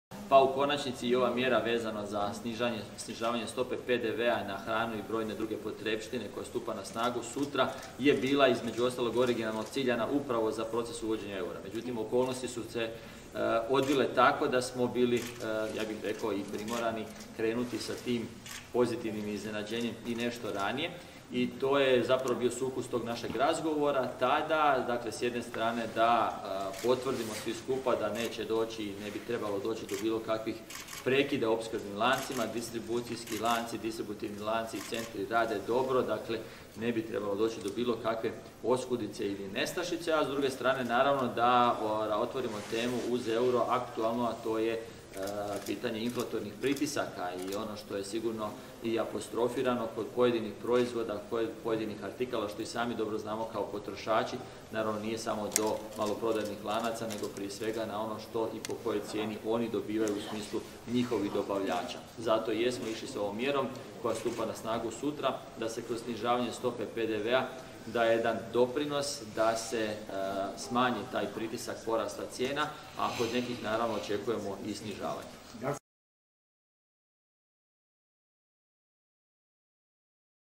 Zdravko Marić: Ne bi trebalo doći do bilo kakvih oskudica ili nestašica
Zdravko Marić, potpredsjednik Vlade i ministar financija dao je izjavu medijima nakon završetka sjednice Vlade Republike Hrvatske u Nacionalnoj i sveučilišnoj knjižnici.